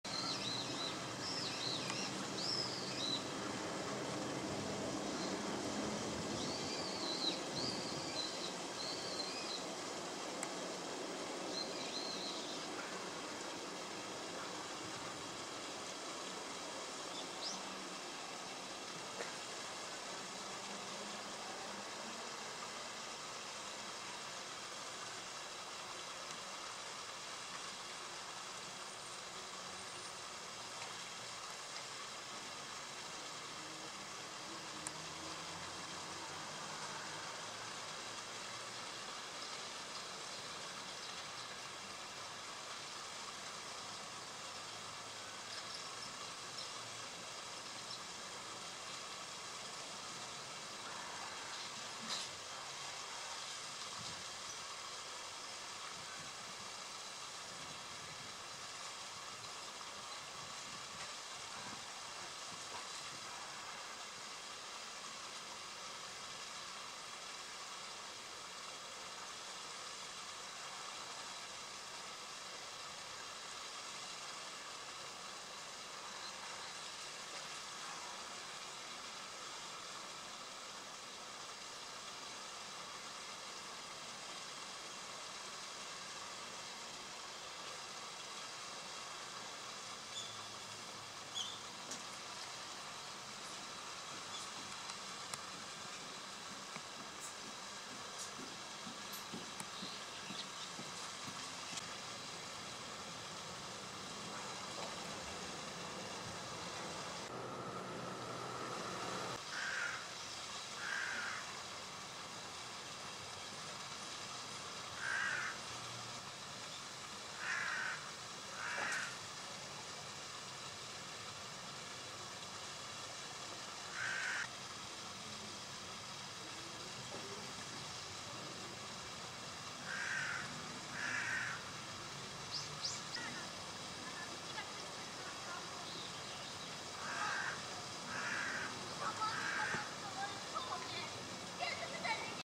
ムビる00227 カワセミ②スカイダイブ10連発超だ kawasemi
kawasemi カワセミ 川蝉 ：Alcedo atthis：Common Kingfisher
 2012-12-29：In Tama River 多摩川にて